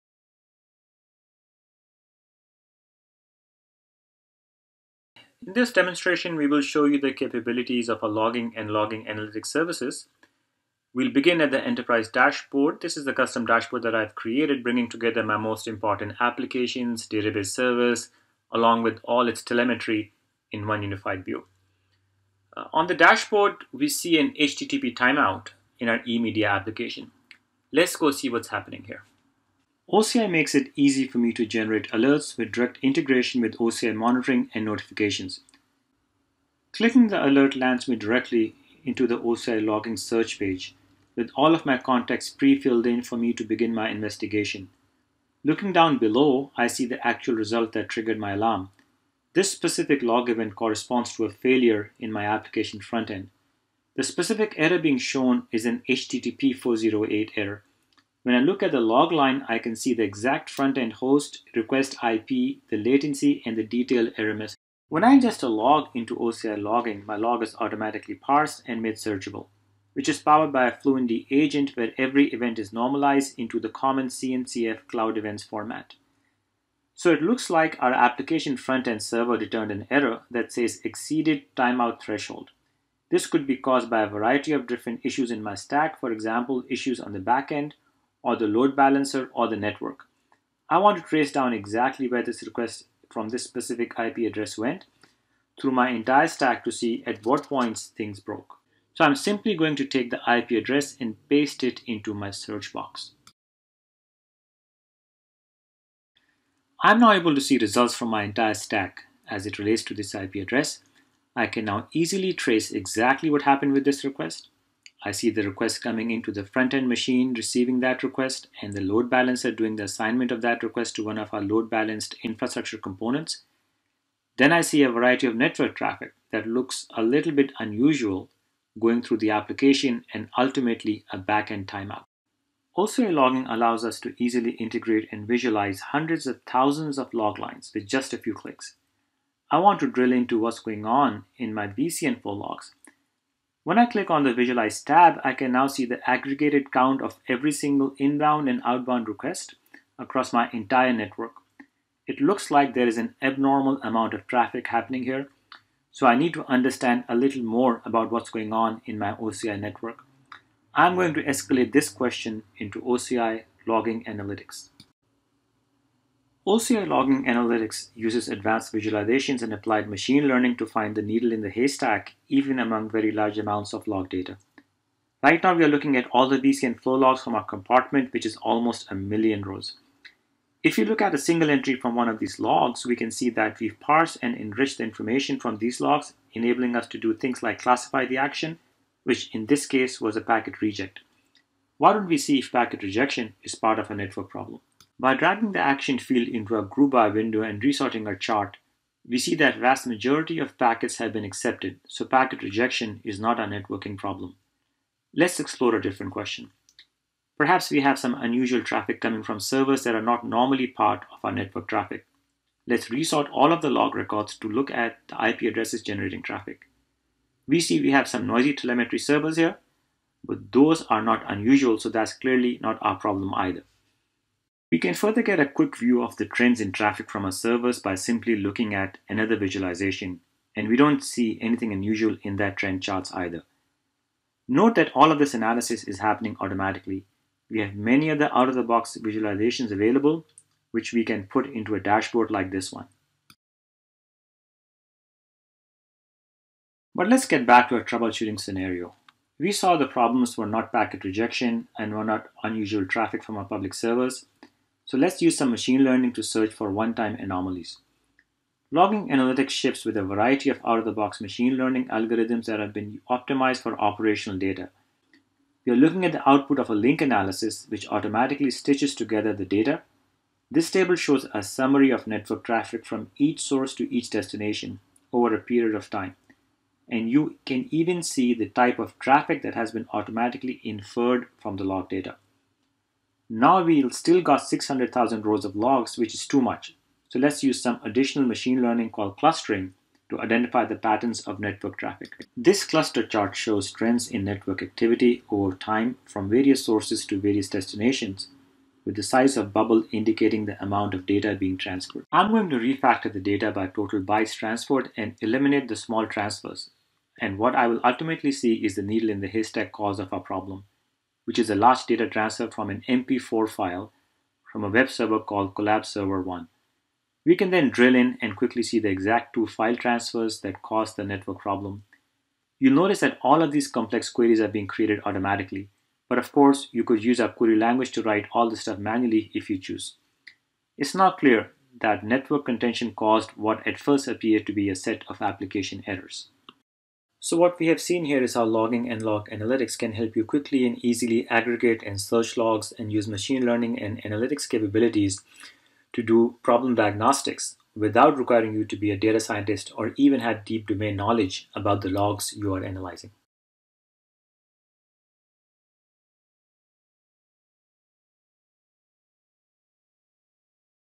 Demo: Logging and Logging Analytics services
In this demonstration, we'll show you the Logging and Logging analytics capabilities of the Oracle Cloud Observability and Management Platform.

for more information.